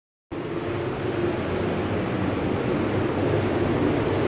Base coat on custom guitar 🎨
Not your average job to come into the shop but sometimes we got some custom work and I’m glad. It’s a nice change of pace sometimes. Hope you guys enjoy! 😁